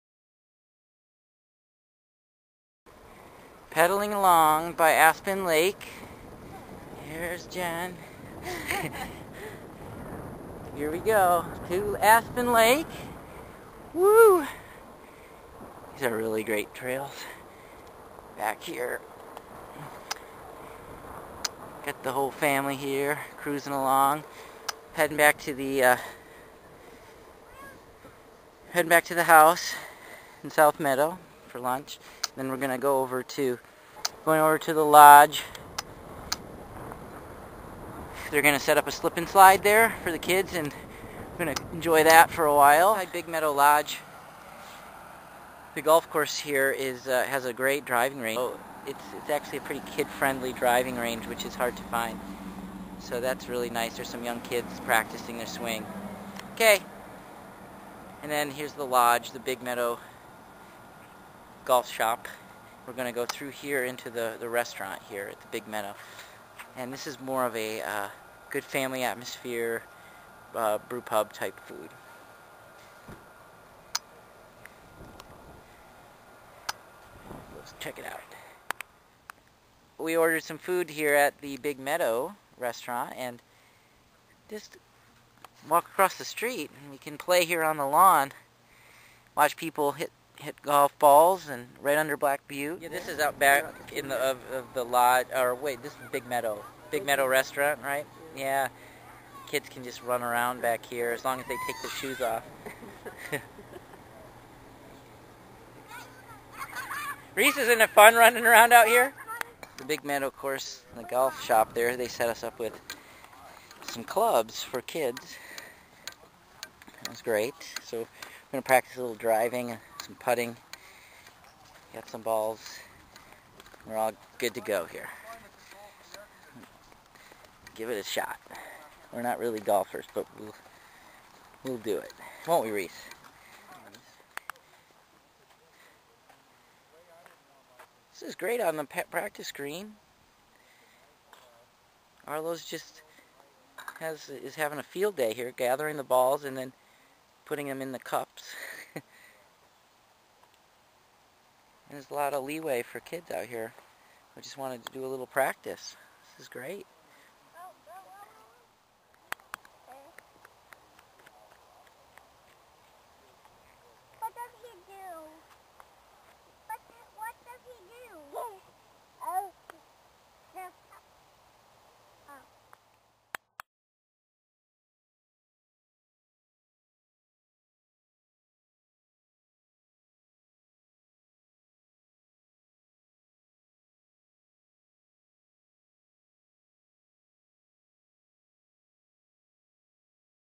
Black Butte Ranch, Part 2
This clip focuses on the Big Meadow golf course and what a great destination it is for kids.